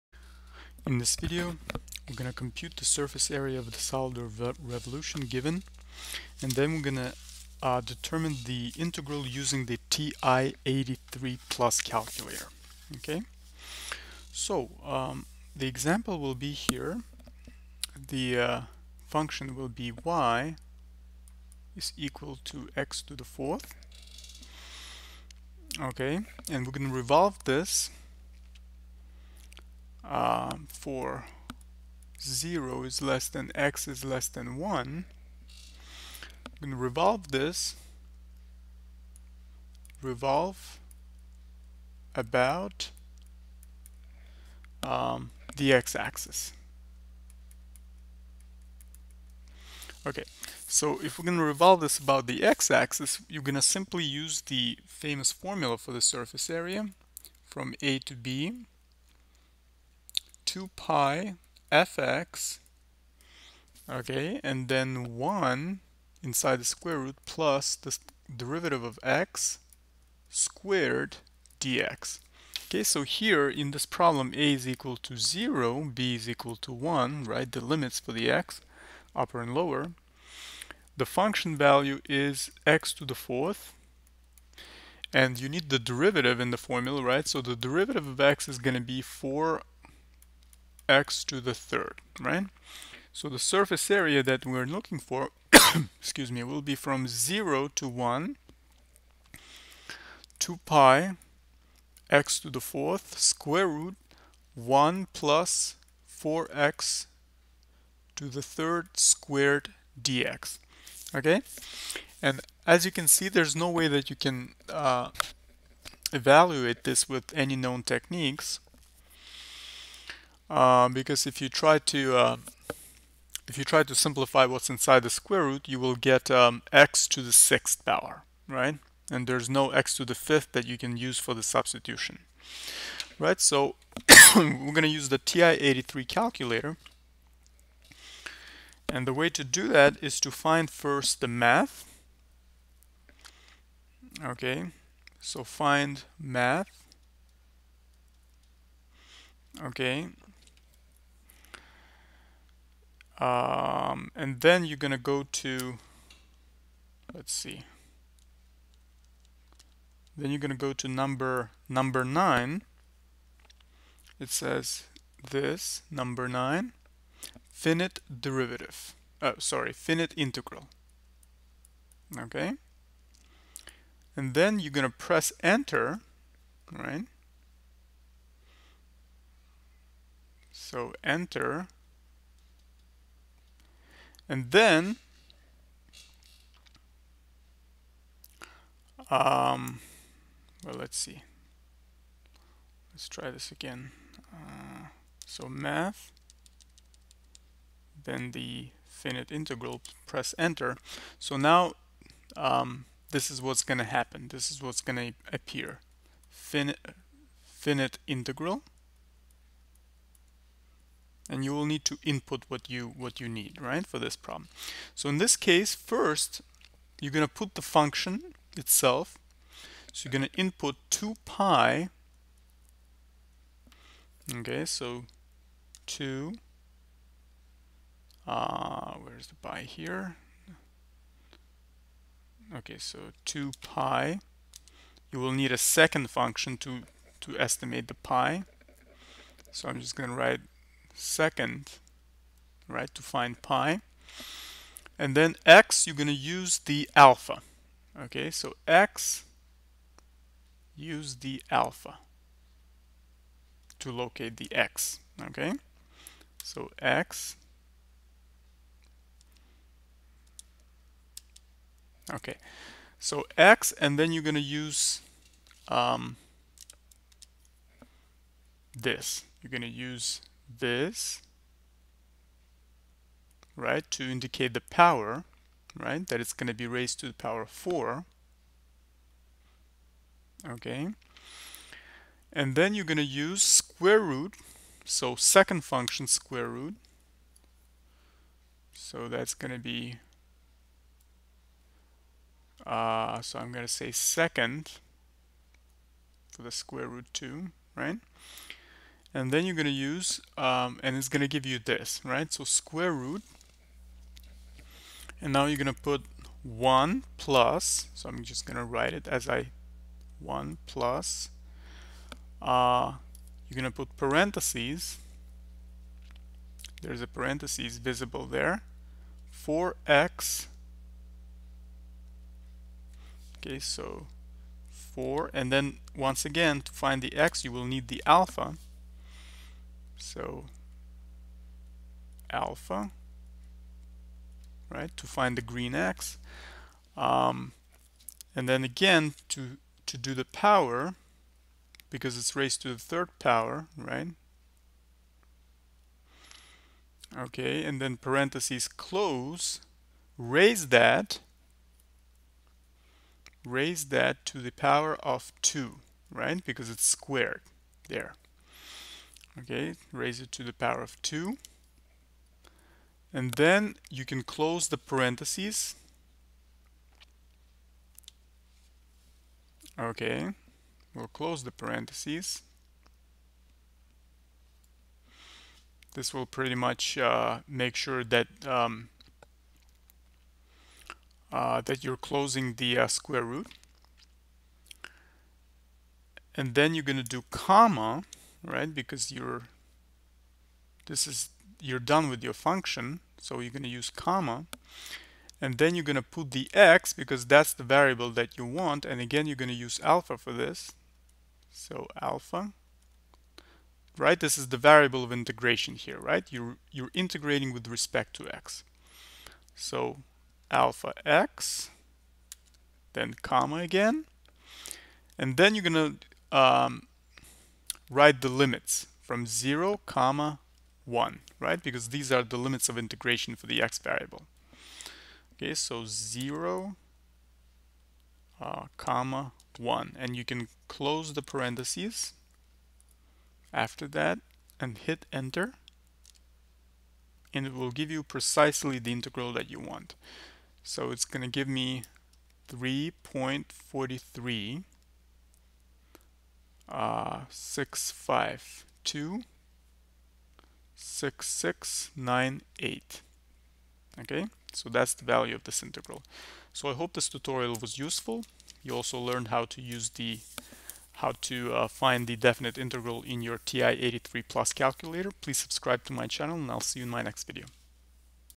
∫ Integration: Surface Area Formula problem ! ! ! ! !
In this video you will learn how to find the Surface Area of a solid generated by revolving the curve y=x^4, from x=0 to x=1, about the x-axis.

0:56 Formula for Surface Area
1:12 Finding components needed for the formula
1:36 Writing and solving the Definite Integral using TI 83 Plus calculator